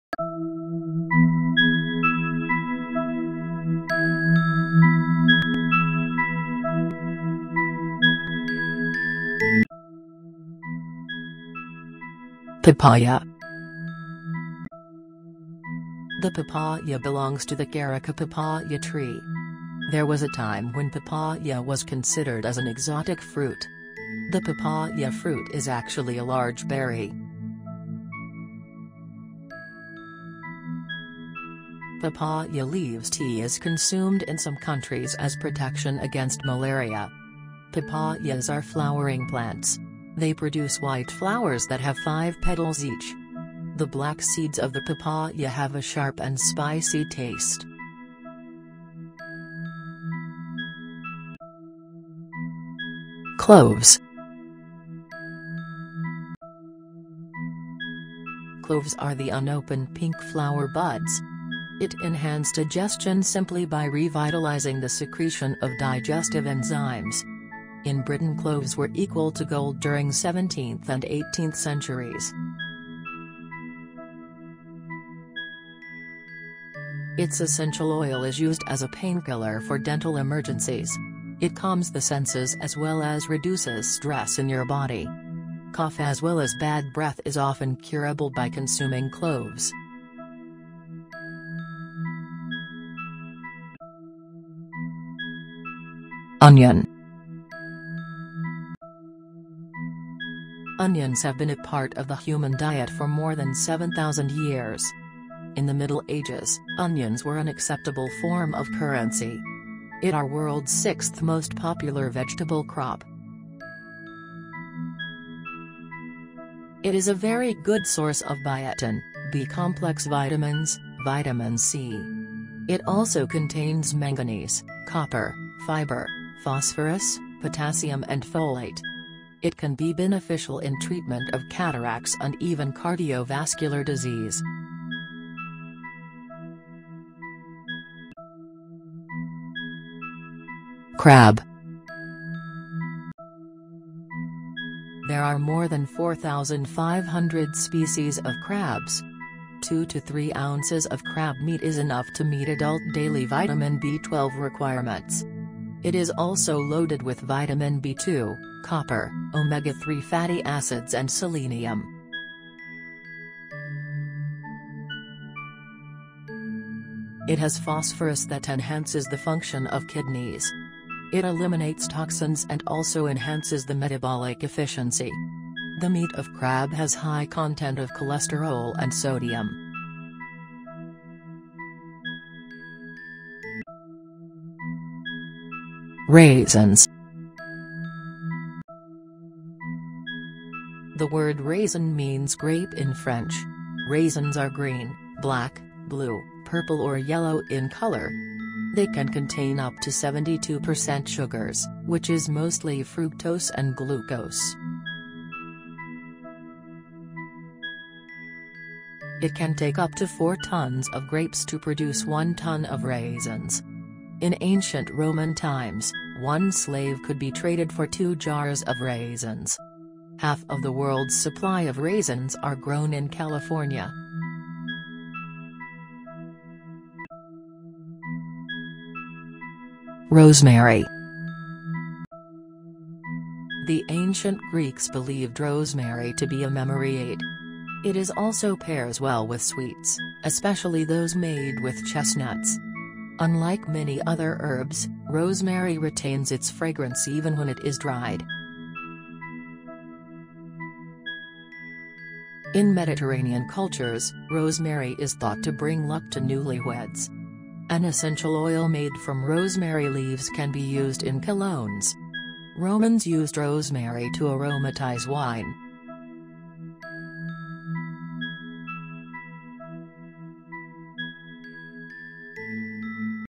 Interesting food facts Part 4
Time Track :
0:00 - Intro
0:11 - Papaya
0:52 - Cloves
1:35 - onion
2:19 - crab
3:04 - Raisins
3:48 - Rosemary